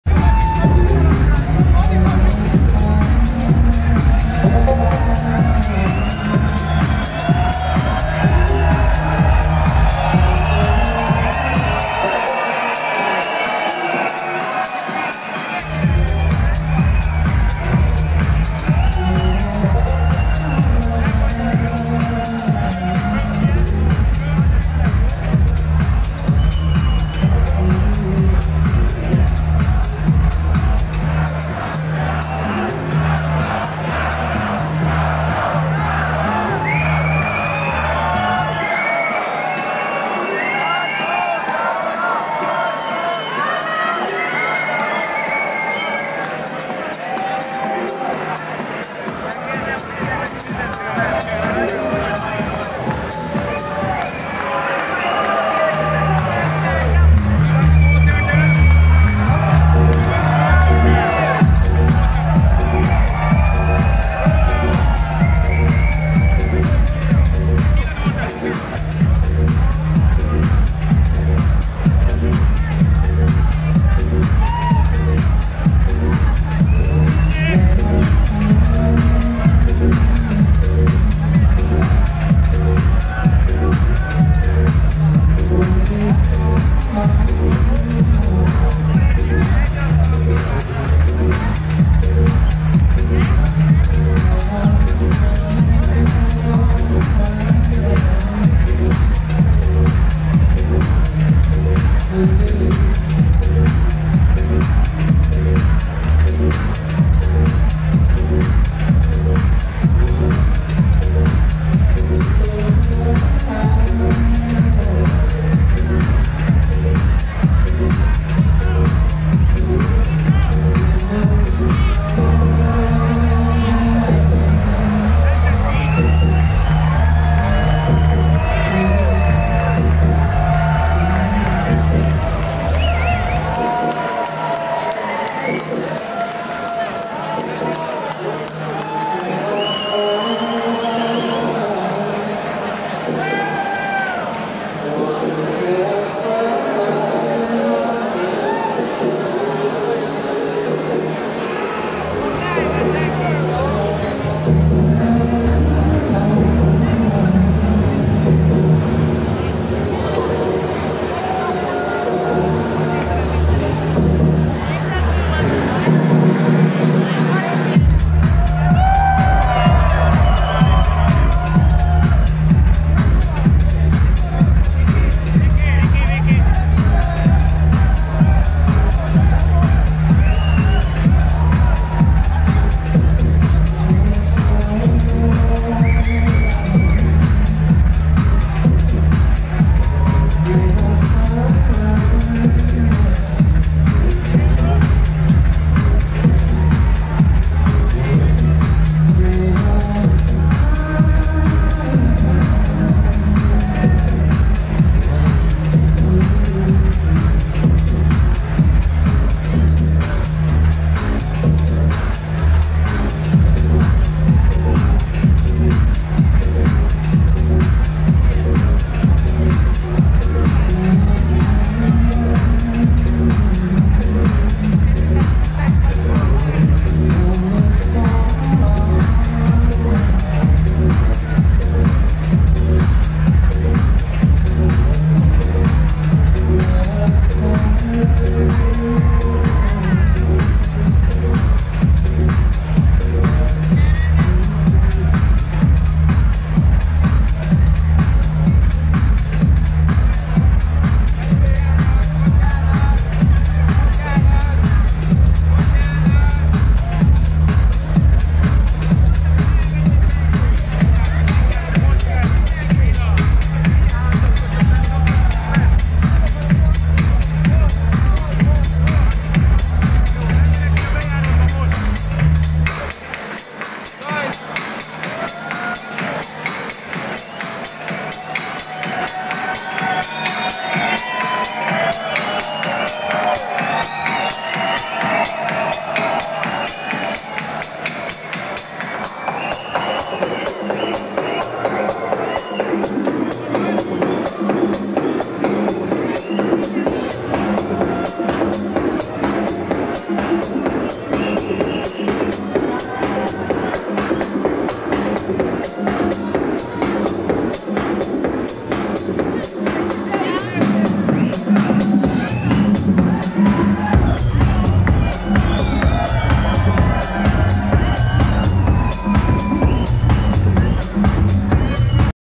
MARCO CAROLA Budapest Cinema Hall 2021-10-02 Last track before the Boss - Unknown (James Cole Edit)
5 min. video of my last track before MARCO CAROLA
Venue: Budapest Cinema Hall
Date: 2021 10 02
ID: My new Unknown - Untitled track (James Cole Edit)